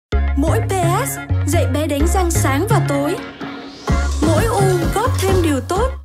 P/S - BẢO VỆ NỤ CƯỜI VIỆT NAM
Cười lên Việt Nam ơi! Vì nụ cười luôn là điều quý giá nhất, nên P/S mang sứ mệnh “Bảo vệ nụ cười Việt Nam", cùng cả nhà và các bé tạo lập thói quen chăm sóc răng và ngừa sâu răng với chiến dịch "Đánh răng sáng và tối" thông qua các hoạt động bổ ích. Mỗi ngày được nhìn nụ cười tươi của cả nhà chính là niềm vui và động lực của P/S đấy!